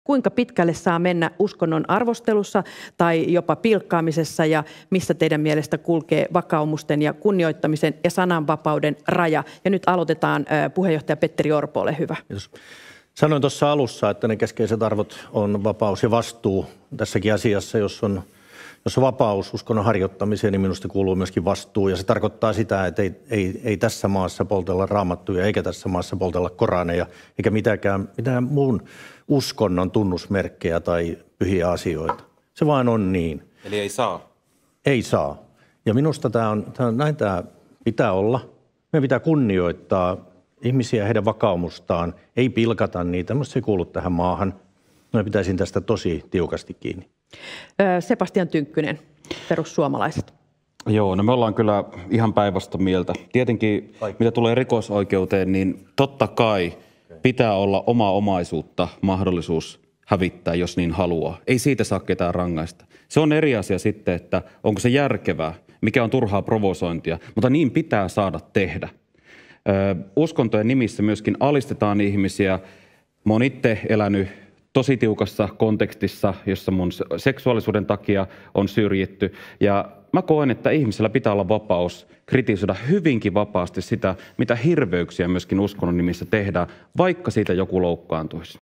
Orpo hiiltyy - Tynkkynen opettaa Kokoomusta sananvapaudesta
Kokoomuksen puheenjohtaja Petteri Orpo ja perussuomalaisten varapuheenjohtaja Sebastian Tynkkynen ottivat yhteen sananvapaudesta kirkon järjestämässä puheenjohtajien arvokeskustelussa.

Mitä enemmän Orpo on hukassa sananvapauden oikeasta merkityksestä, sitä jämäkämmin hän yrittää julistaa asiaa: näin se vaan on!

Tynkkynen antaa nopean kertauksen sananvapaudestaja ja muistuttaa samalla kokoomuksen puheenjohtajaa siitä, kuinka omistamaansa omaisuutta pitää saada hävittää ilman rikosoikeudellisia seuraamuksia.

MUUT SOMEPROFIILIT:
TikTok (125k): @sebastiantynkkynen
Instagram (30k): @sebastiantynkkynen